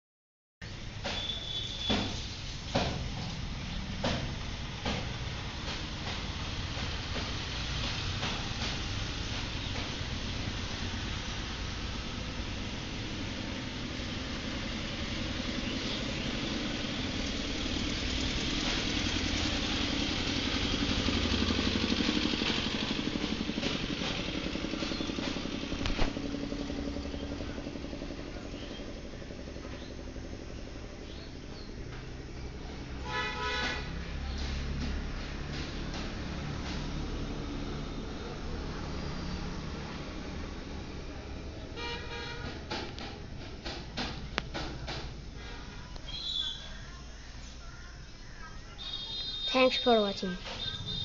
Water Spinach Plant overview
Water Spinach is a leafy plant and delicious to eat.It is also called as kalmi plant